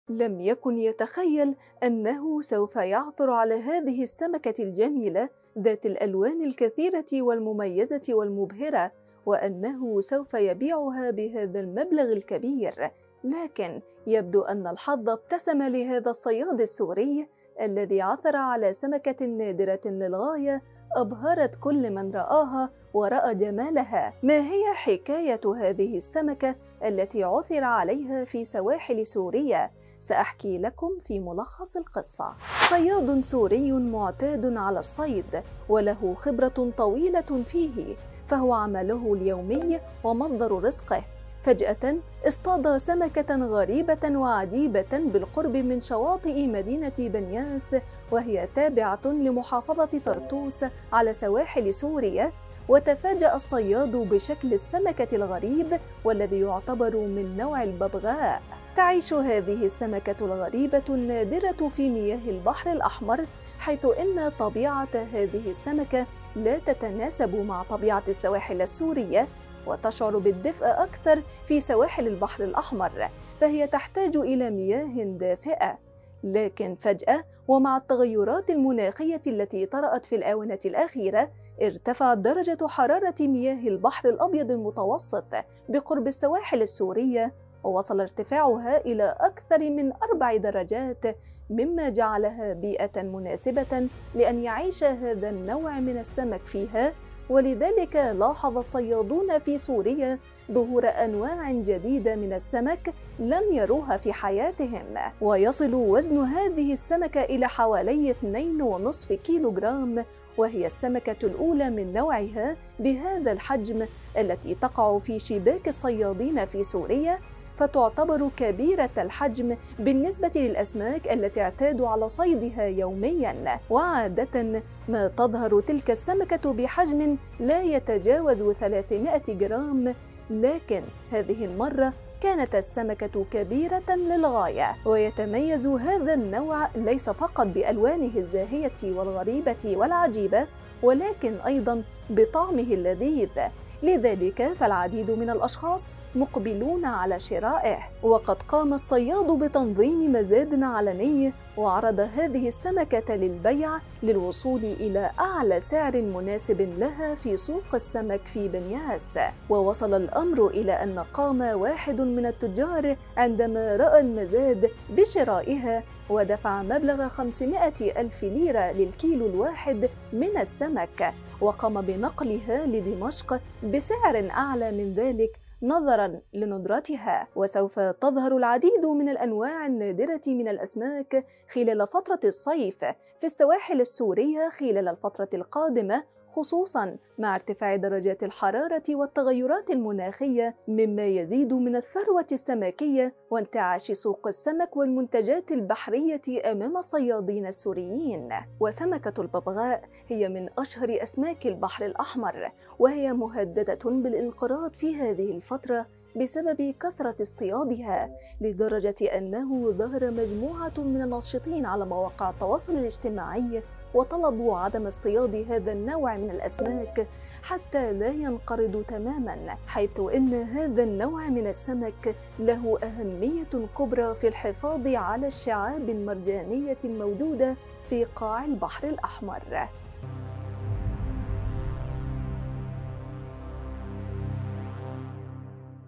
صياد سوري يصطاد سمكة نادرة ويبيعها بسعر خيالي. لن تصدق سعر الكيلو منها. وكيف وصلت إلى سواحل سوريا ؟
تعيش هذه السمكة الغريبة النادرة في مياه البحر الأحمر حيث أن طبيعة هذه السمكة لا تتناسب مع طبيعة السواحل السورية وتشعر بالدفء أكثر في سواحل البحر الاحمر فهي تحتاج إلى مياه دافئة ، لكن فجأة  ومع التغيرات المناخية التي طرأت في الآونة الأخيرة ارتفعت درجة حرارة مياه البحر الأبيض المتوسط بقرب السواحل السورية ووصل ارتفاعها إلى أكثر من أربع درجات .